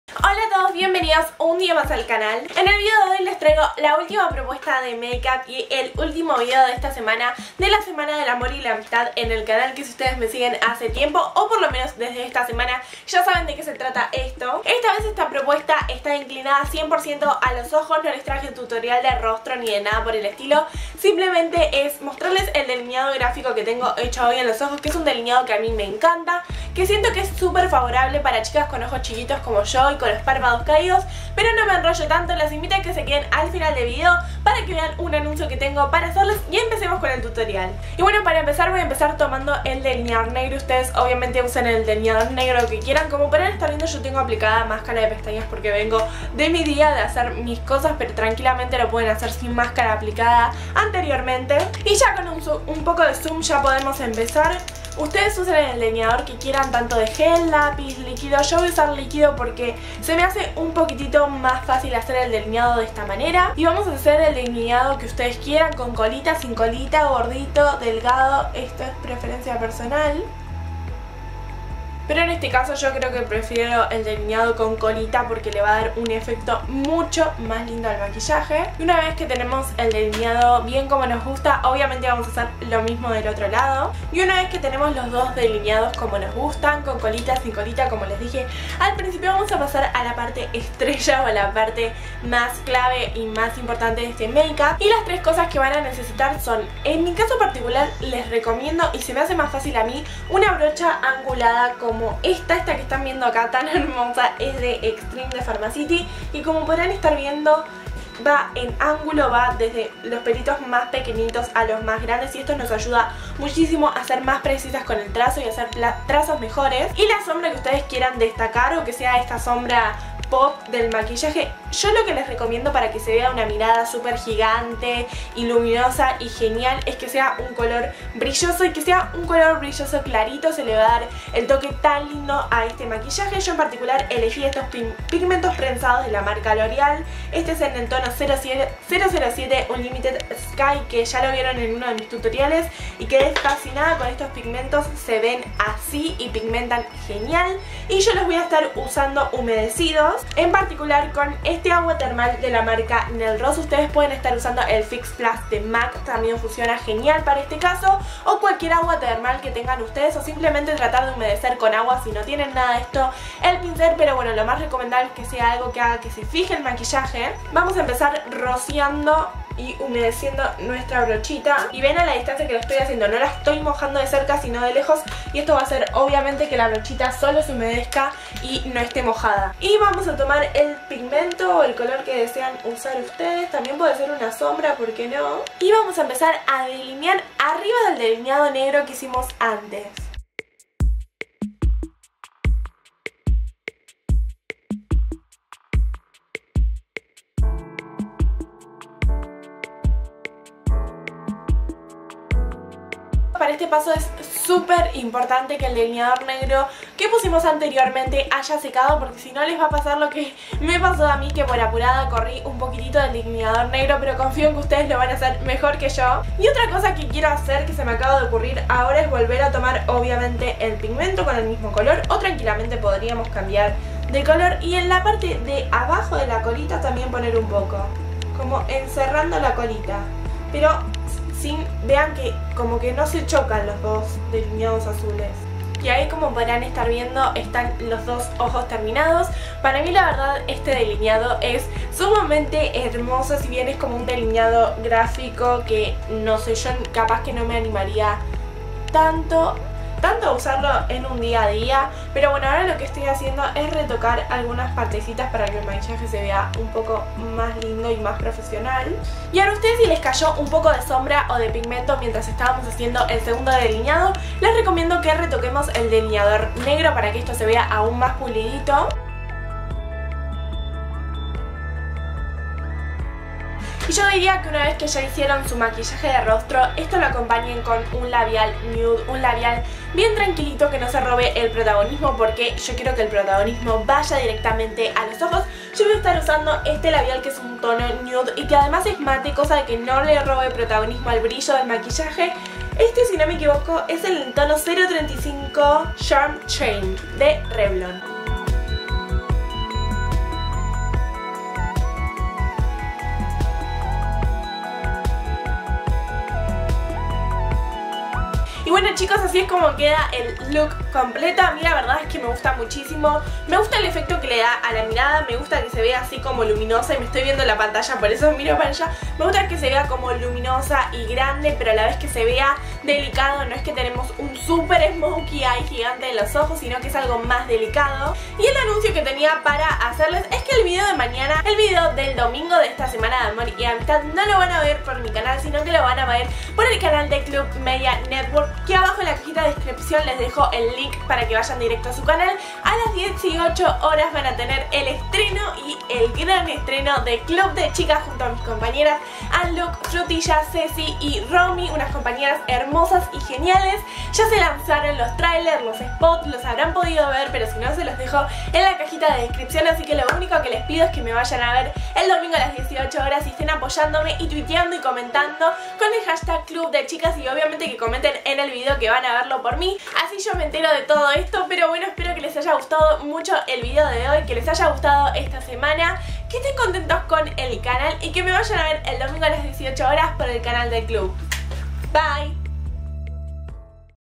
SemanaDeLaAmistad GRAPHIC LINER Tutorial | Julieta Berea
Terminando la SemanaDeLaAmistad y un tutorial de un delineado grafico super facil!

VIDEO EN HD!

TODO MODA      Pink love Liner
LOREAL PARIS  002 Unlimited Sky
EXTREME           Brocha angulada Pink Collection
NELL ROSS        Thermal Water

Domingo 18:00 hs. Estreno CLUB DE CHICAS por el canal del Club! Los espero a todos ahi, twitteandome con el hashtag ClubDeChicas para estar mas en contacto

Juli.